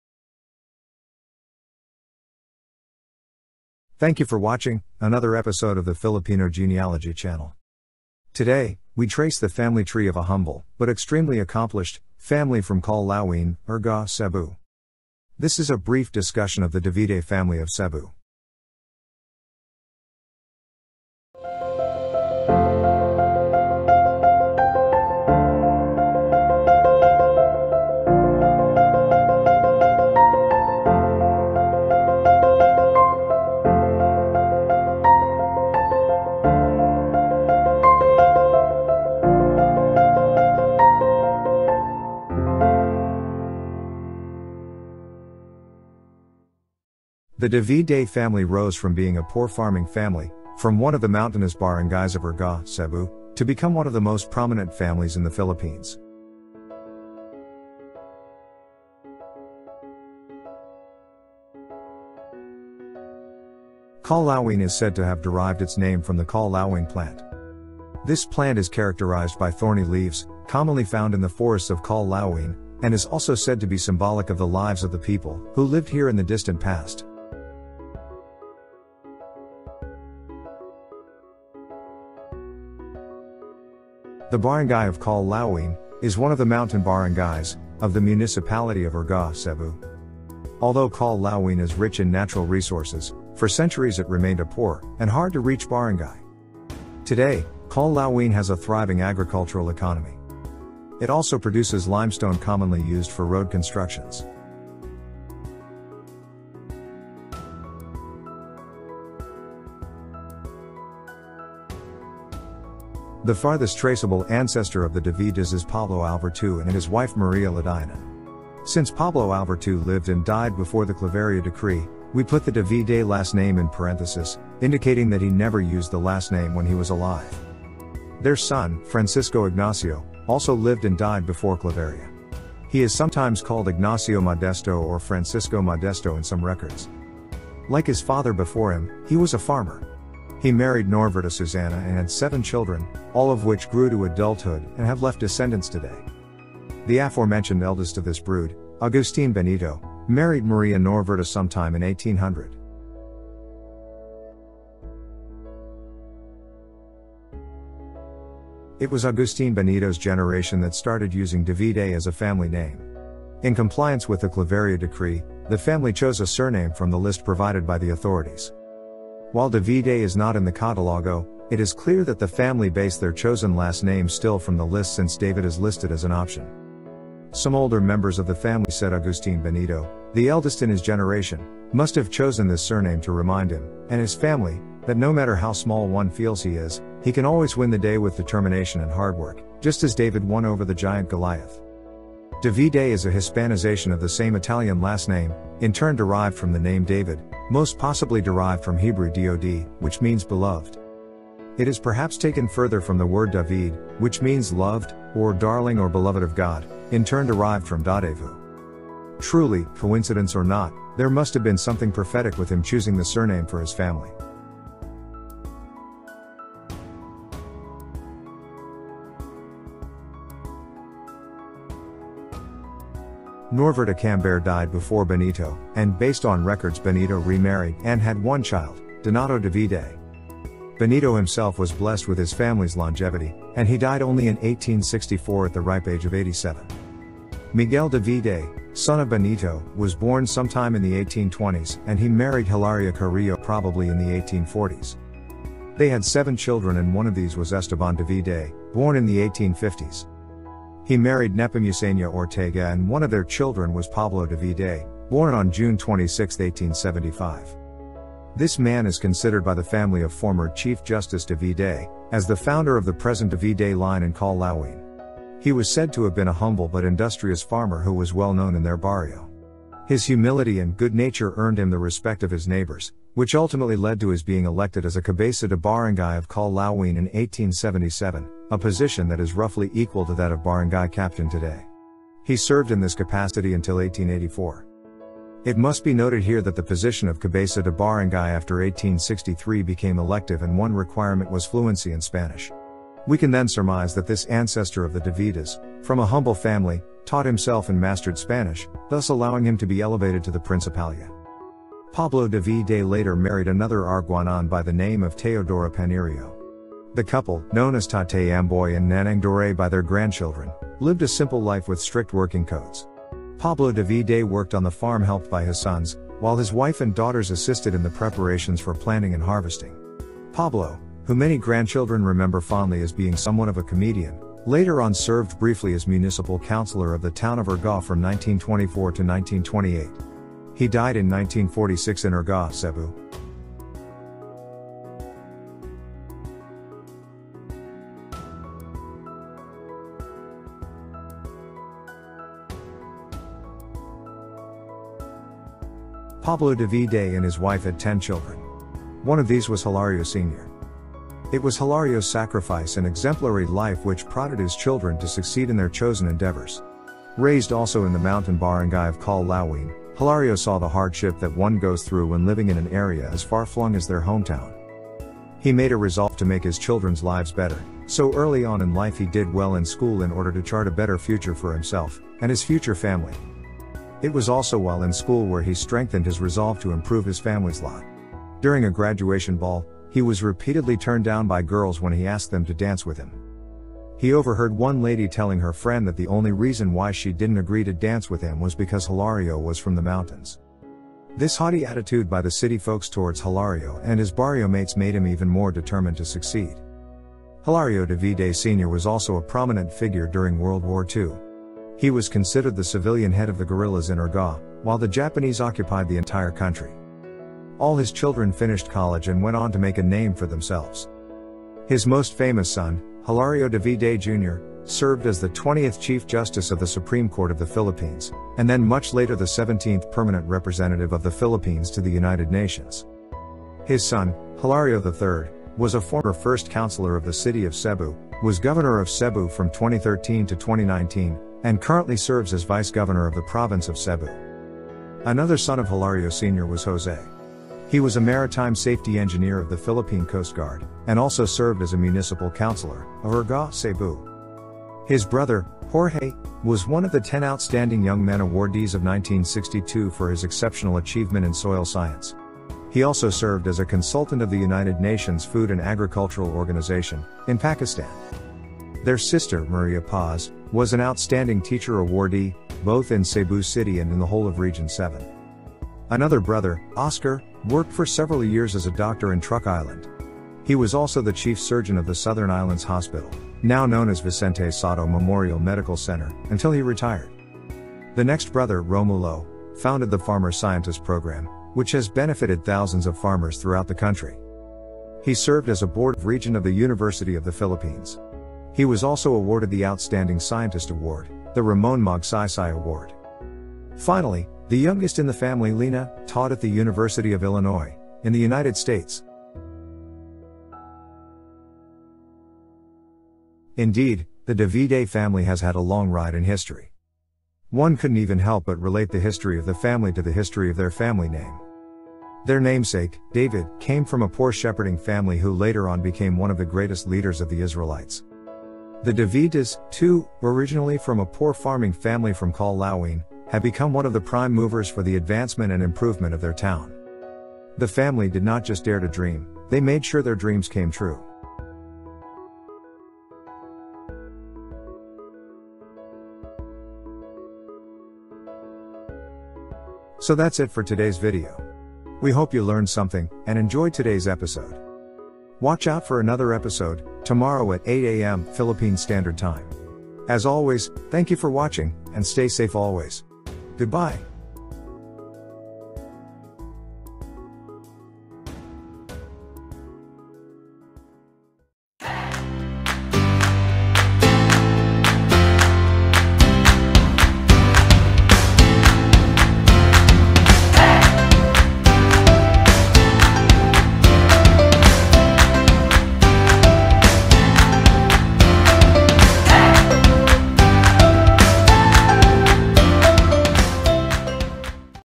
Filipino Family Tree | The Davides of Argao, Cebu
Today, we trace the family tree of a humble, but extremely accomplished, family from Colawin, Argao, Cebu. This is a brief discussion of the Davide family of Cebu.

The Davide  family rose from being a poor farming family from one of the mountainous barangays of Argao, Cebu, to become one of the most prominent families in the Philippines.

CREDITS:
Chart/Script: T. Sales
Narration/Editing: T. Sales
Intro animation: Filipino Genealogy Channel